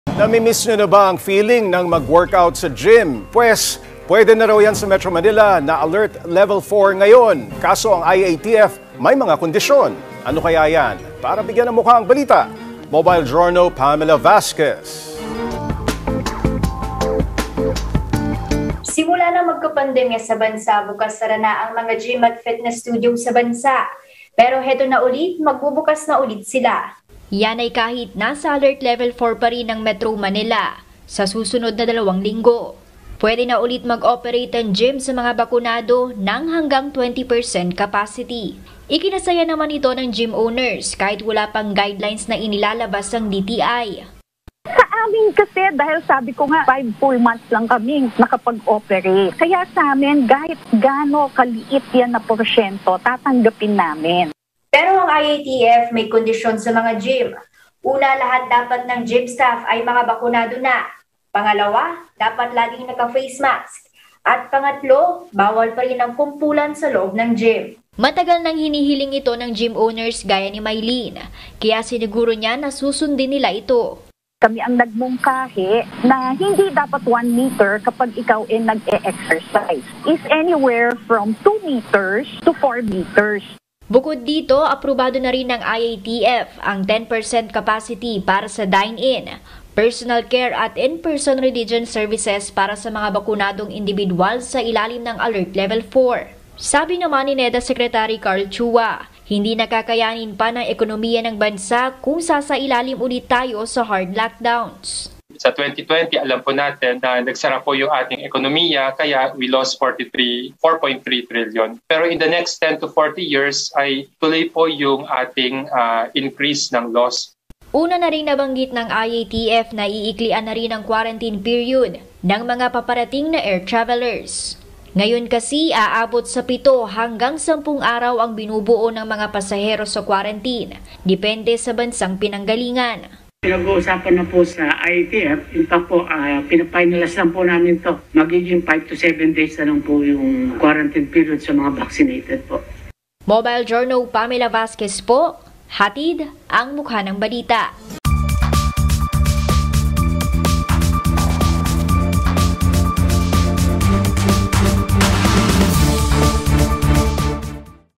Gyms, pwede na mag-operate sa Alert Level 4
OneBalitaPilipinas | Balik-operasyon na ang mga gym sa Metro Manila sa ilalim ng Alert Level 4.

Alamin ang IATF guidelines para sa pagbubukas ng gyms at iba pang establishment. | via MOJO Pamela Vasquez